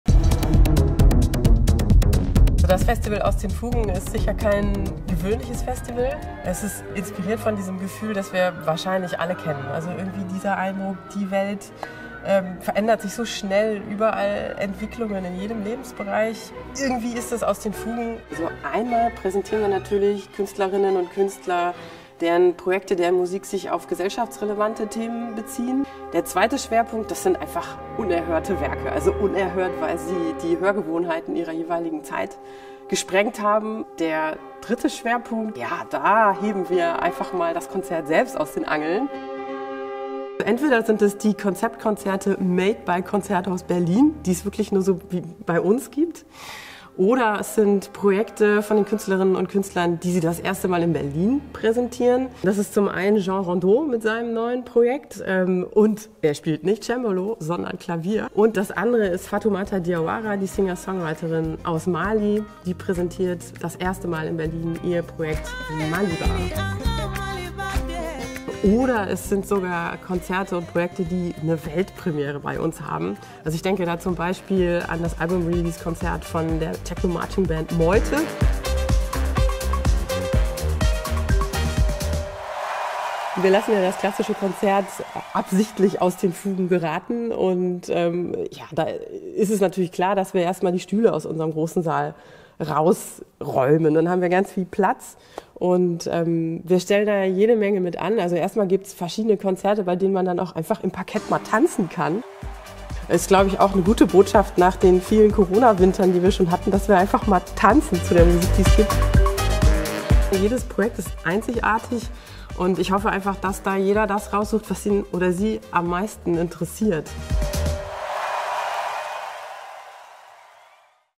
Aus den Fugen | Festivalteaser | Konzerthaus Berlin
Zwei Wochen "Aus den Fugen" 💥 Visionäre Werke, die Scharniere zwischen Epochen bilden, 🎻Musiker*innen, die auf eine Welt aus dem Lot reagieren und Formate, die den normalen Ablauf im Konzertsaal aus den Angeln heben. Programmdirektorin Dorothee Kalbhenn zum Festival vom 14. bis 27. November 2022 im Konzerthaus Berlin.

Zu Gast sind unter anderem Fazil Say (Klavier), Cuarteto Casals, Jonathan Stock­hammer (Dirigent), Singer-Songwriterin Fatoumata Diawara & Band, ensemble reflektor, Ensemble des Podium Festivals Esslingen, Cédric Pescia (Klavier), Jean Rondeau (Cembalo) und Tancrède Kummer (Percussion), Diana Tishchenko (Violine), Benjamin Appl (Bariton), Žilvinas Brazauskas (Klarinette) und Martynas Levickis (Akkordeon).

Video: Markus Stein